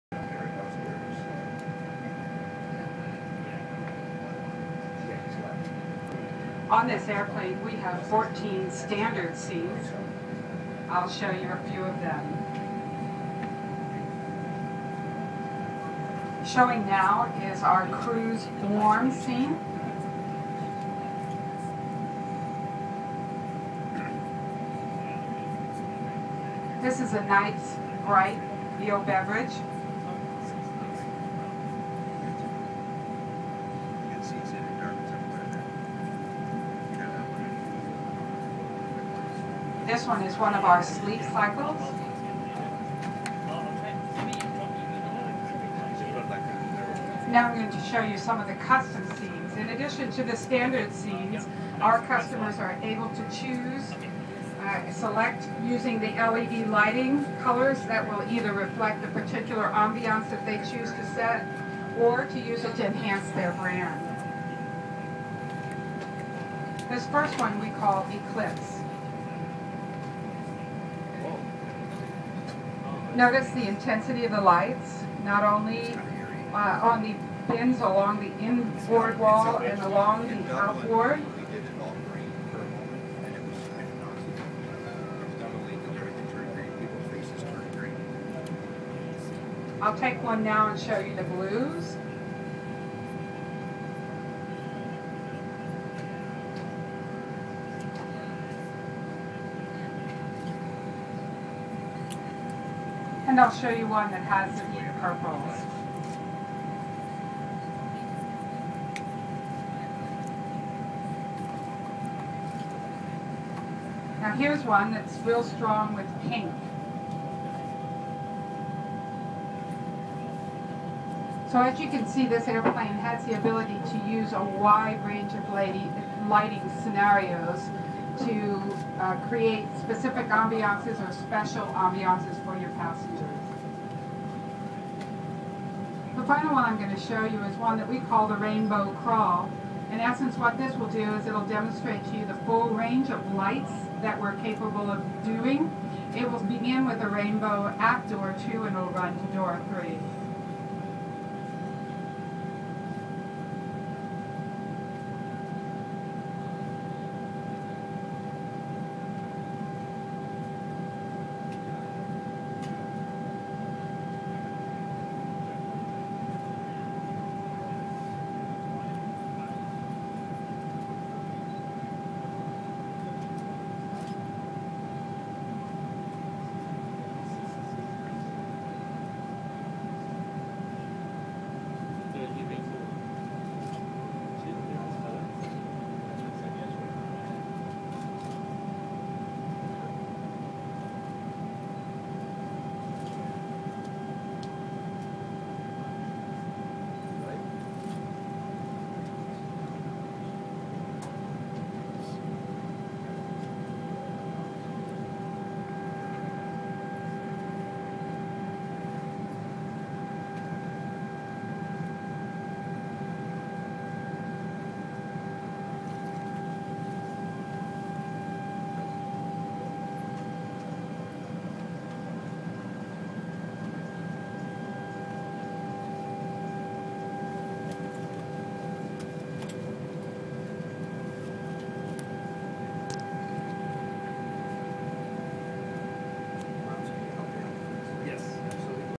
Boeing 787 Dreamliner LED Lighting System
Mood lighting demonstration on the Boeing 787 Dreamliner.